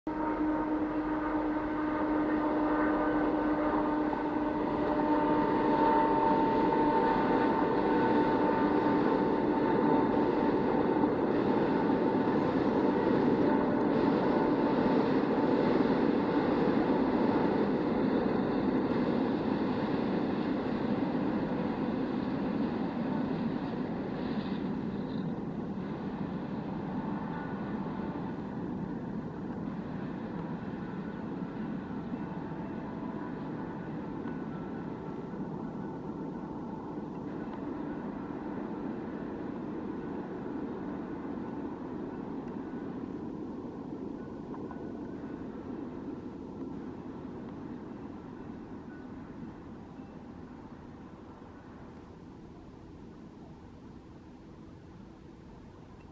Comboio Internacional n° 89160 pelo verde Minho.
Locomotiva: Ibercargo 5038
Data: 25/07/2024
Hora: 18:49
Local: Silva, Linha do Minho
Serviço: Valença → Ramal Celbi